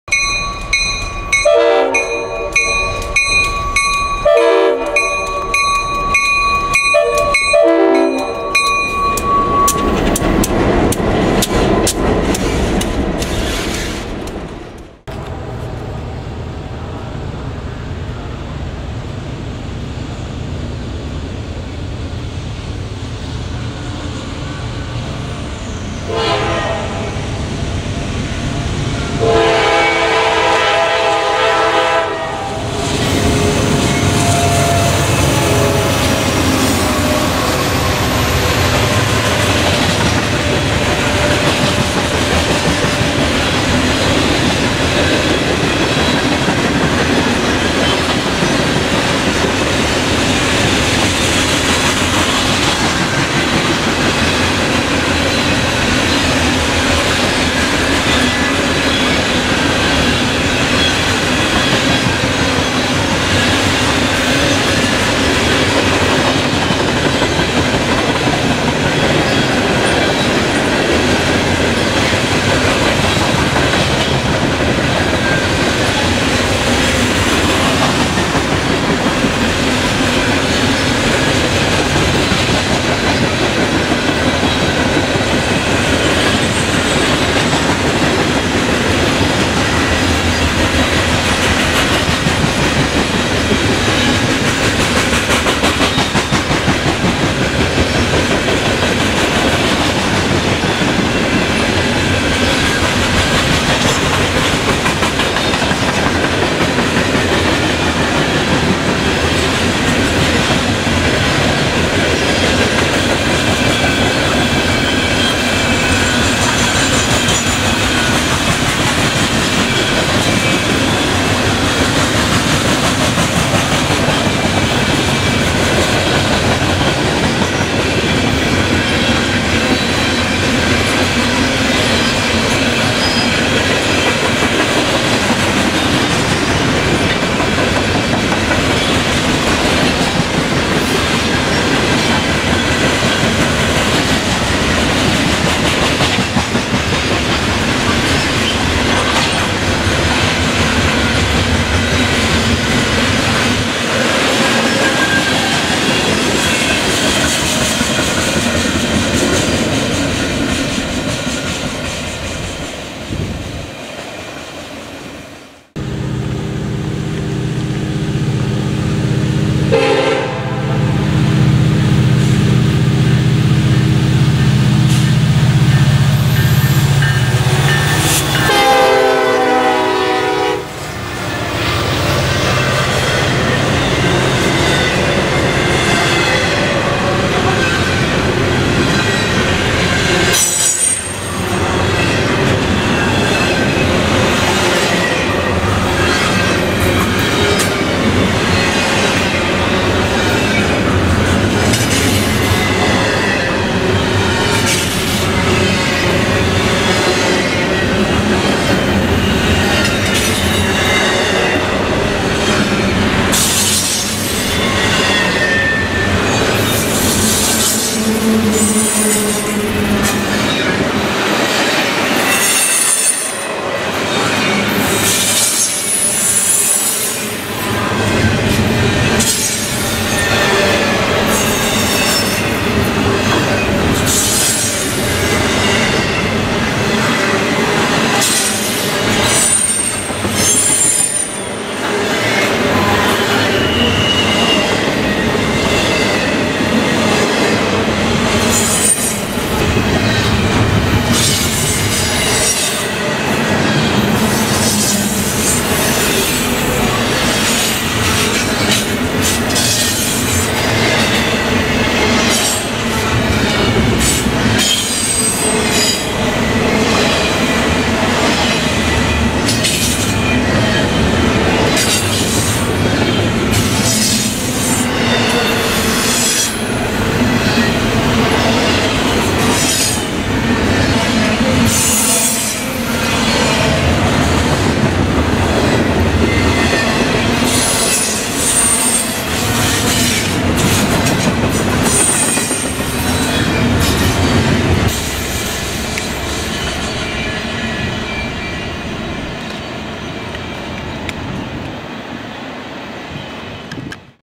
Fast NS Coal Empties and SD70M-2 Leading Fast Doublestacks
I've been trying to upload videos for a long time and it finally worked...
Went to the Emsworth Locks and Dam today, and here the first two of seven trains I saw. Part two will have the other five trains and will be more of a "professional" looking production :)
The first train was coming through just as we were parking, and the second one surprised me.